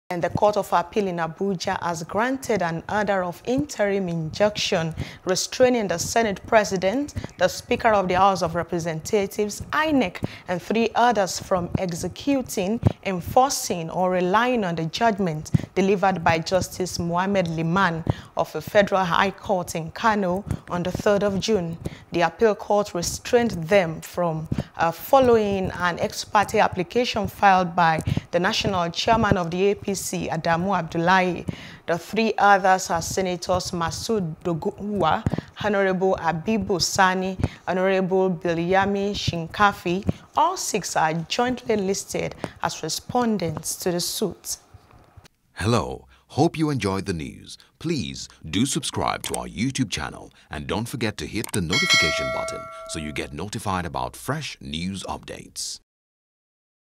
Statutory Delegates: Court Restrains INEC, Lawan, Gbajabiamila On Primary | NEWS
The Court of Appeal in Abuja has granted an order of interim injunction restraining the Senate President, the Speaker of the House of Reps, INEC and three others from executing, enforcing or relying on the judgment delivered by Justice Mohammed Liman of a Federal High Court, Kano, on 3 June, 2022.